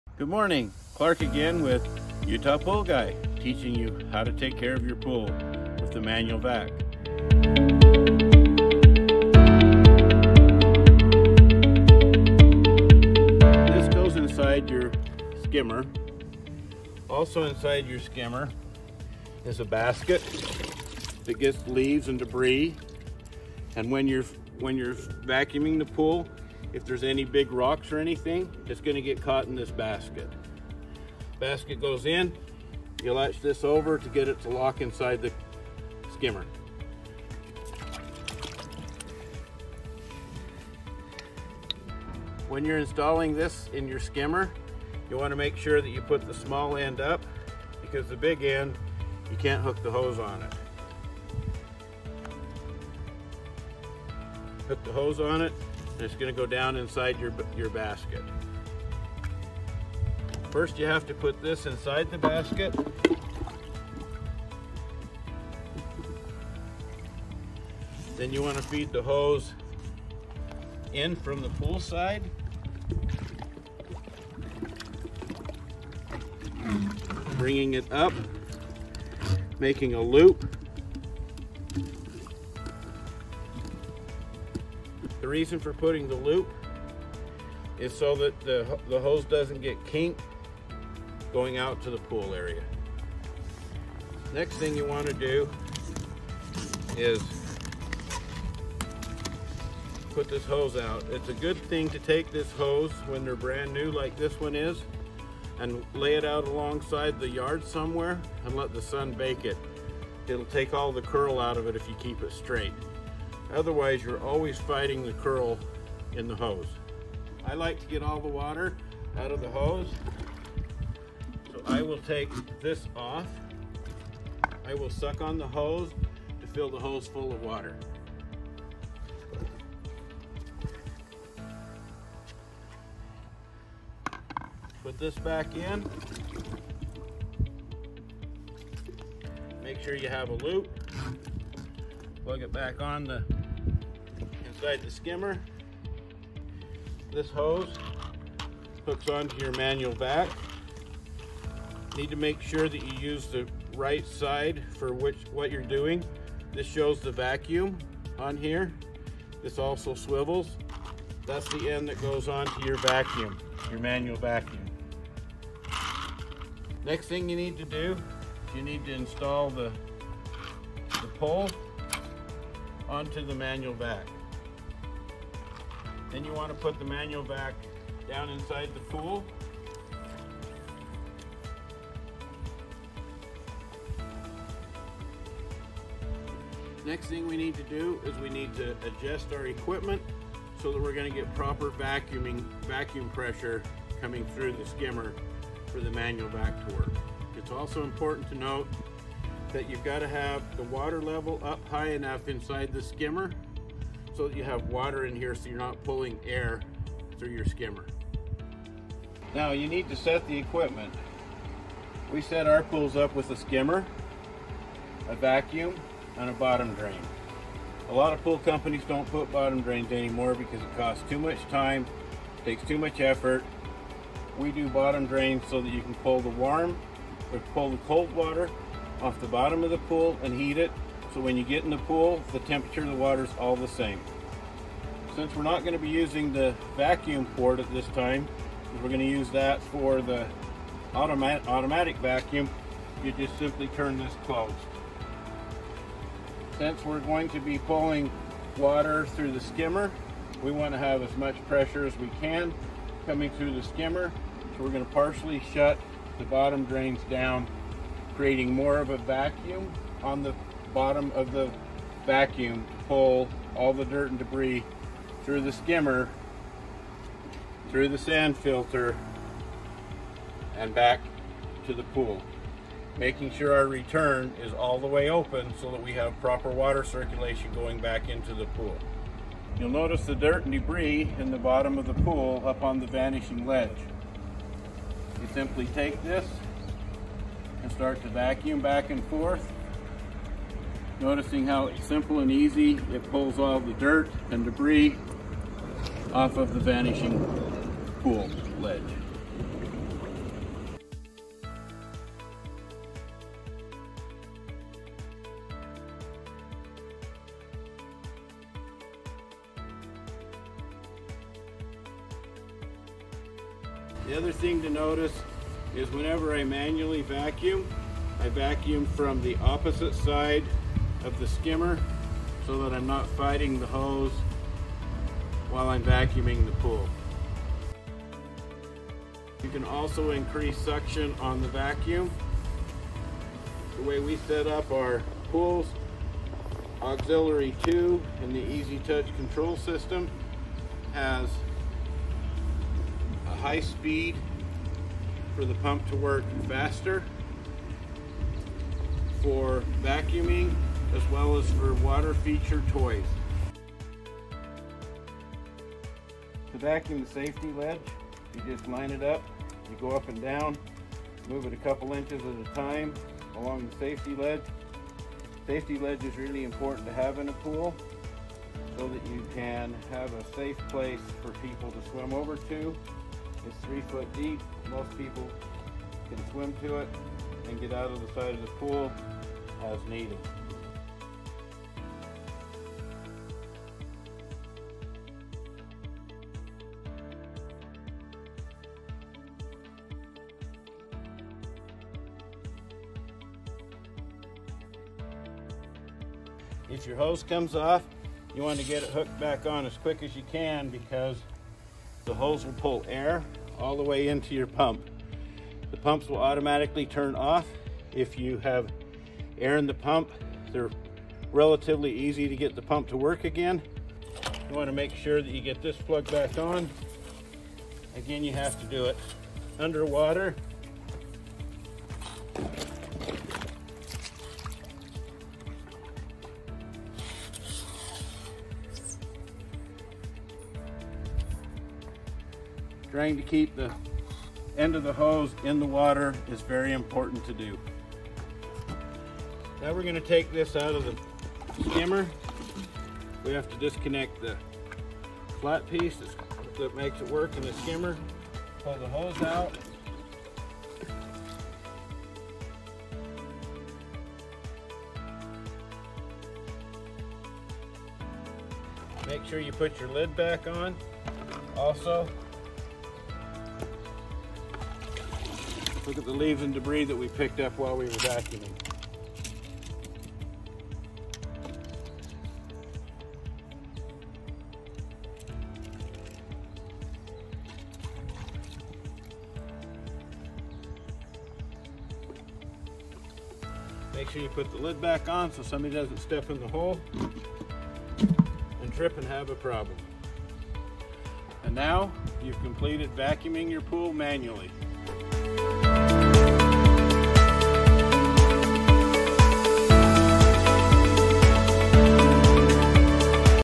How To Manually Vacuum Your Fiberglass Pool | Utah Pool Guy
Wondering how to manually vacuum your new swimming pool? This video will walk you through the process step by step.

If you have additional questions about fiberglass pool maintenance, visitor our